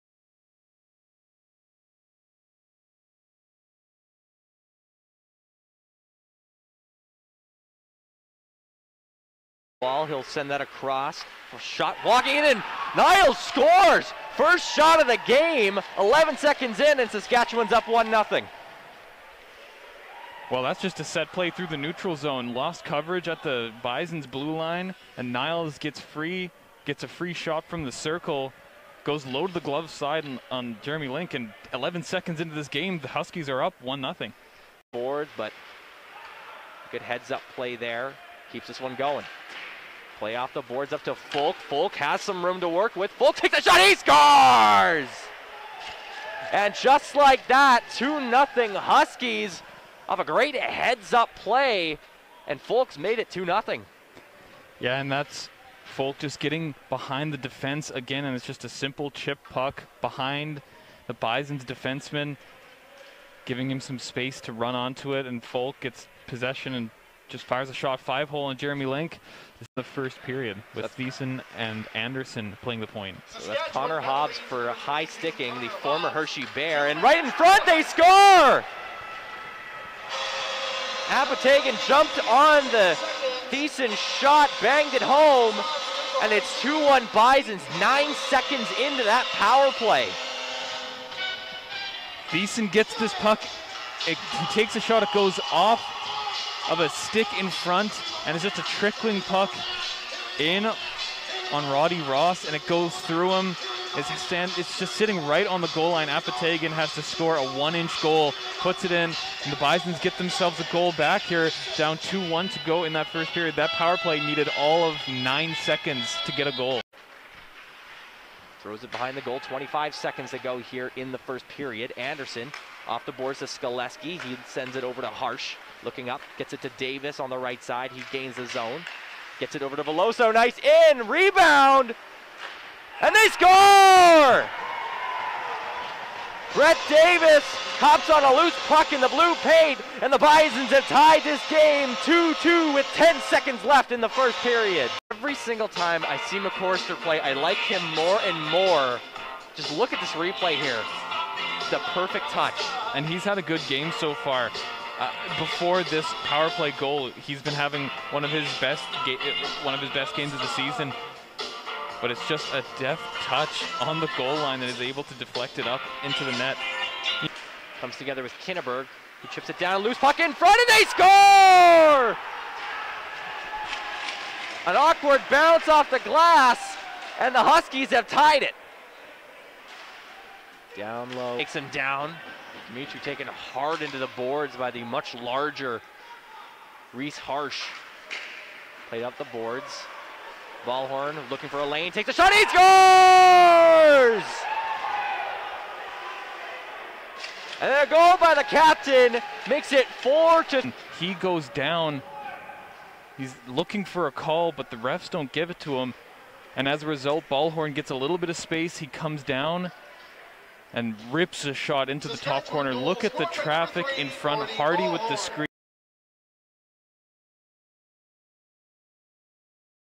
Bisons Highlights | Saskatchewan 4, Manitoba 3 | October 29, 2021
The Manitoba Bisons lost to the Saskatchewan Huskies 4-3 on Friday, October 29 at the Wayne Fleming Arena. Here are the highlights.